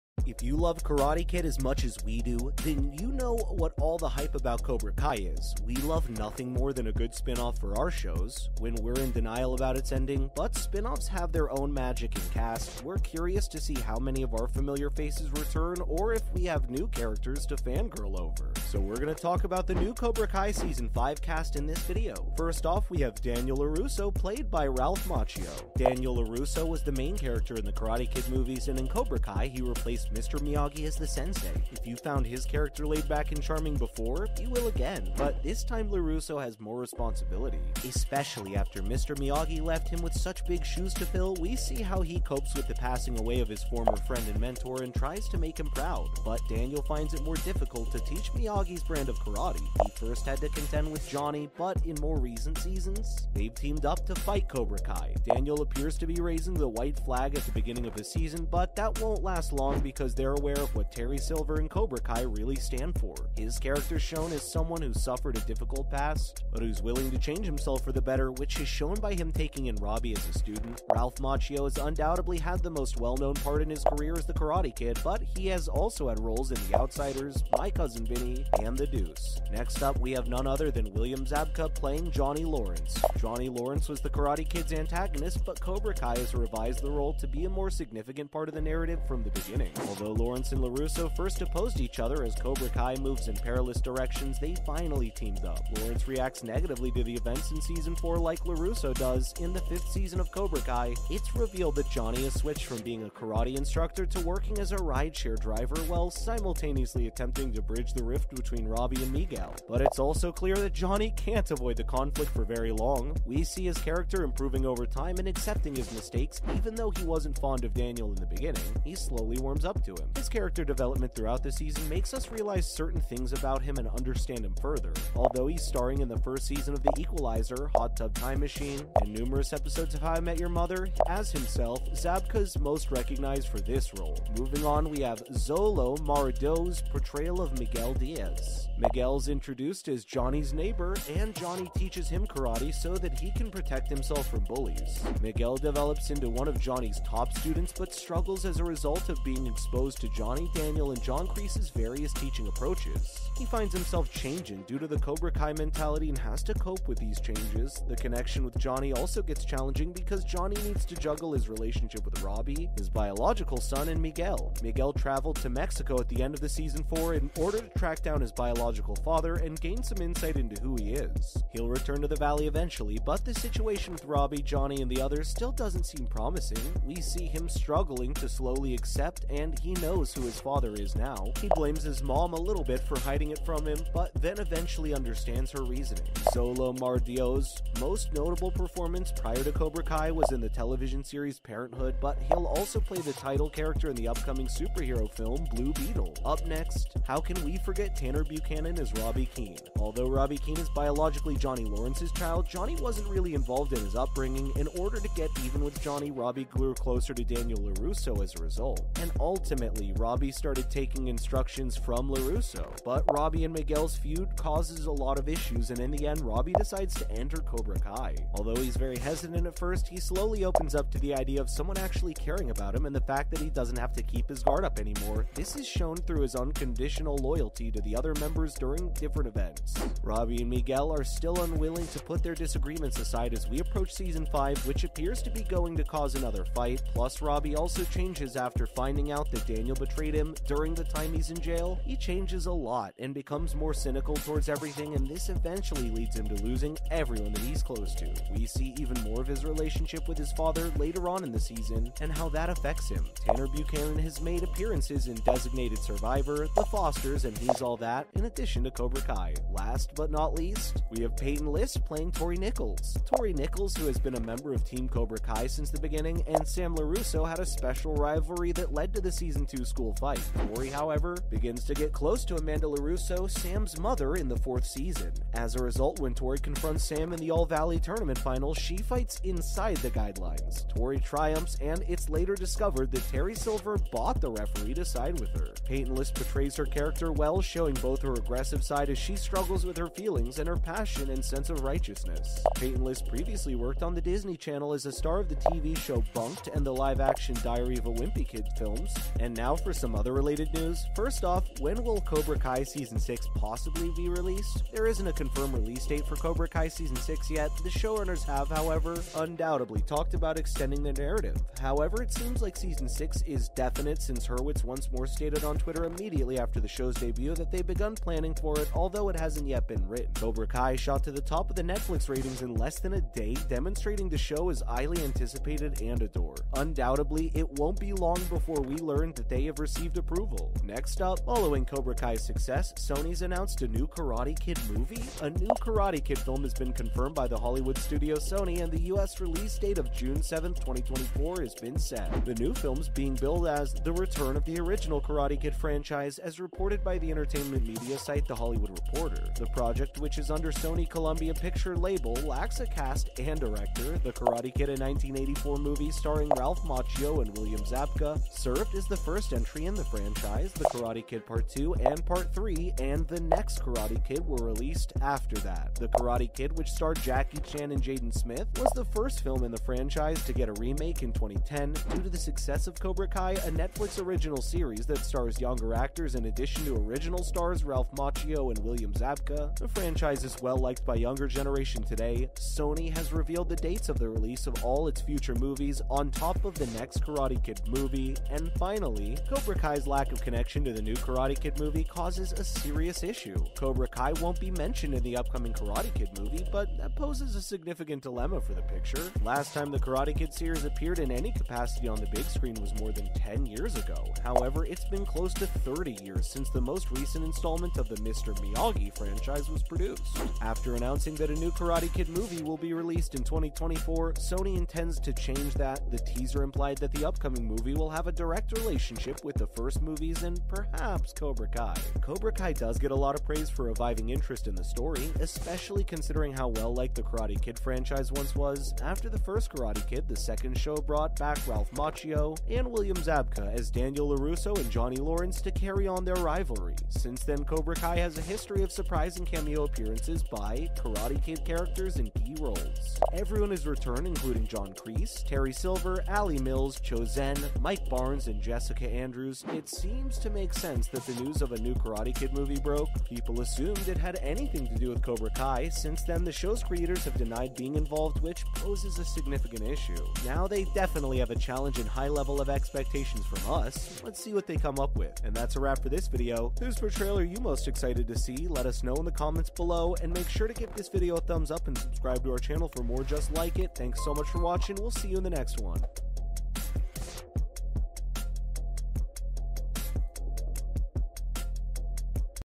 Cobra Kai Season 5 Cast REVEALED..
Welcome back to Drama Central, today on the channel we are going to Meet the Cobra Kai season 5 cast: who’s who in the Karate Kid spinoff series + other related news. With reboots and spinoffs of old TV shows and movies a go-to strategy, Cobra Kai is an example of it firing on all cylinders. Cobra Kai season 5 returns to Netflix September 9, as the spinoff/continuation of the classic Karate Kid movies has satisfied original fans and captured new ones with its mix of comedy and action and the blending of new and returning characters. It’s those characters we’re focusing on here with this rundown of the Cobra Kai season 5 cast. Many of the familiar players from the Cobra Kai and Miyagi-Do dojos are back, facing new challenges. But we’re also getting some brand new players into the epic rivalry, including more with Karate Kid connections. Let’s meet the Cobra Kai season 5 cast:

Ralph Macchio as Daniel LaRusso. Daniel LaRusso was the hero of The Karate Kid movies and in Cobra Kai has taken over the sensei role that Mr. Miyagi had in the movies (RIP Pat Morita). However, Daniel is having a harder time with teaching Miyagi’s style of karate. While he first had to deal with Johnny, the former rivals team up to take on Cobra Kai in the most recent seasons. This season looks to start with Daniel waving the white flag, but not for long, as they know what Cobra Kai and Terry Silver truly stand for. William Zabka as Johnny Lawrence. Johnny Lawrence was The Karate Kid’s villain, but from the start, Cobra Kai has reframed the character to be more of a central part of the story. While Lawrence and LaRusso start off as enemies, they eventually become allies as Cobra Kai goes into dangerous directions. Like LaRusso, Lawrence takes the events of season 4 hard. The trailer for Cobra Kai season 5 shows Johnny has moved on from teaching karate to becoming an rideshare driver, while also trying to mend the relationship between Robby and Miguel. Keep watching for more on Cobra Kai season 5 cast.